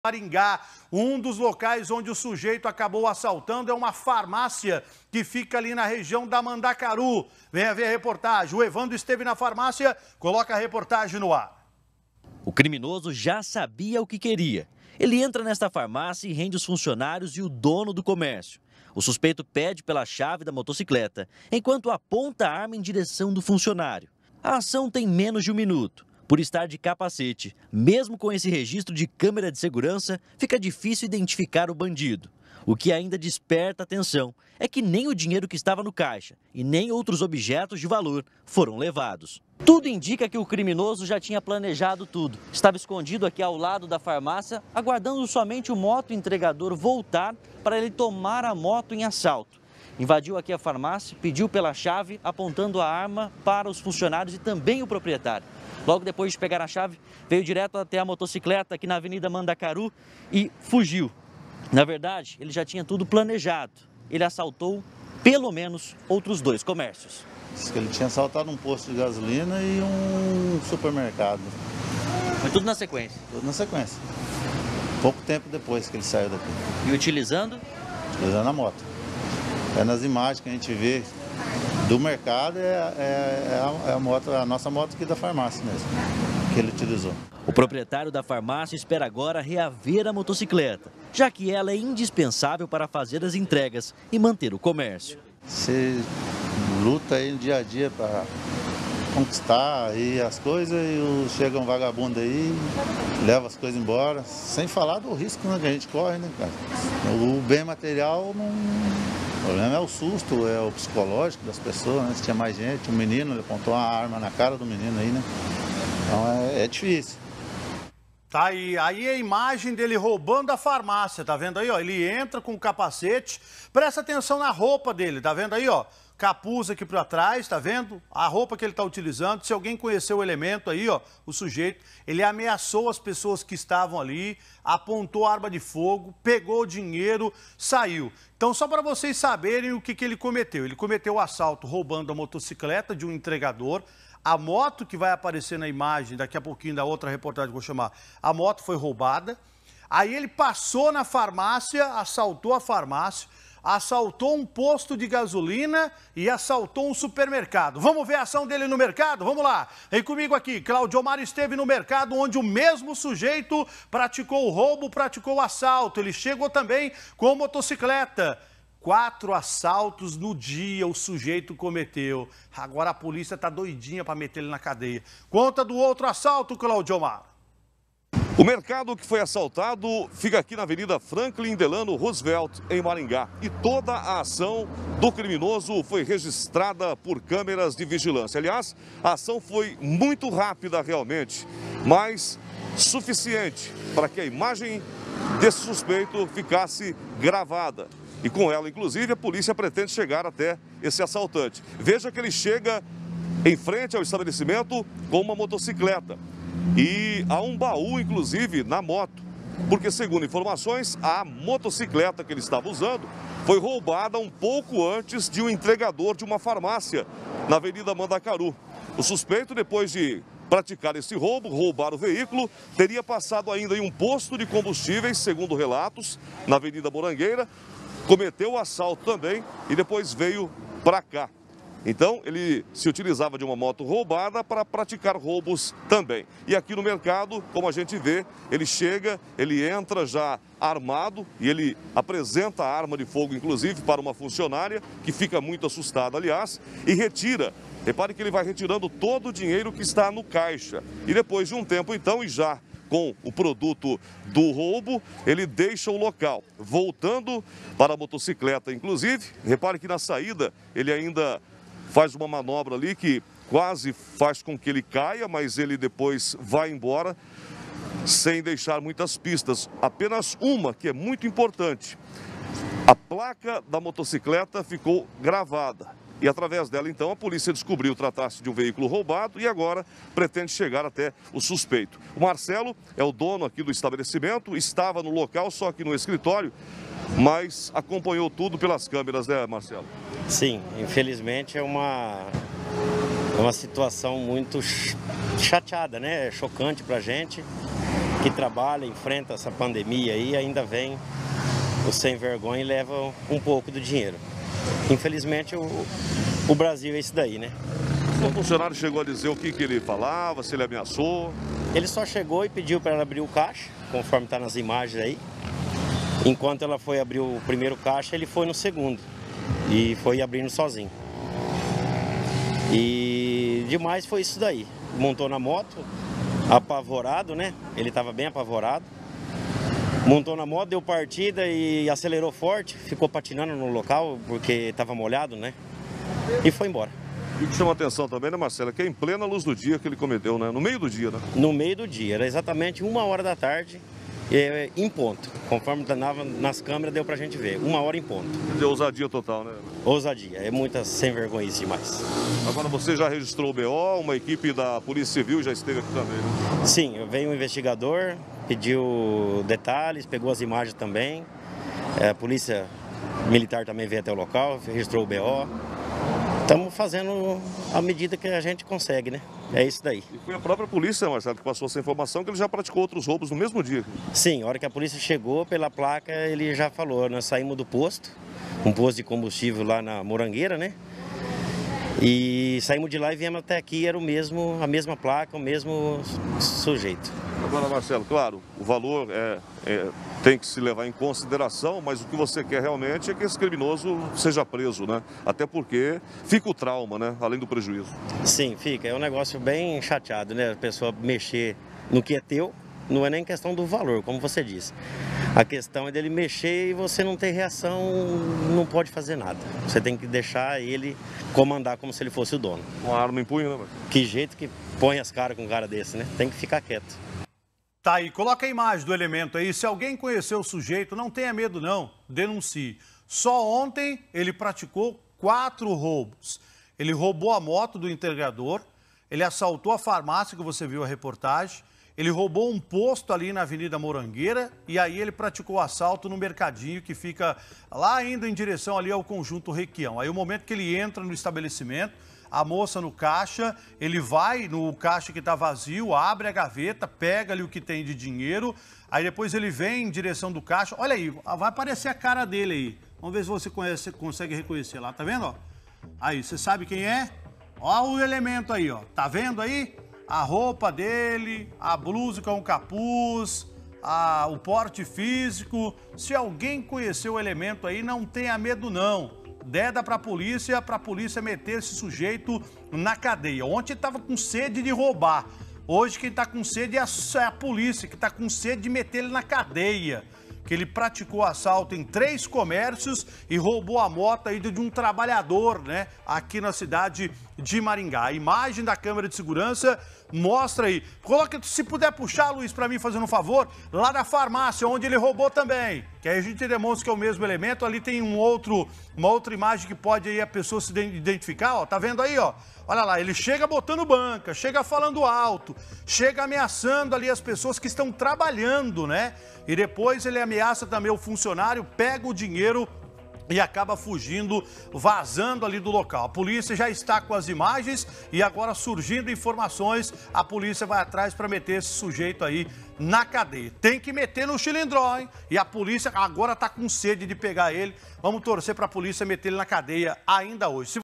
Bandido furta moto e sai fazendo assaltos em Maringá
Ele usou uma farmácia para furtar uma moto. Agora o dono do estabelecimento quer reaver o prejuízo. Confira todos os detalhes na reportagem!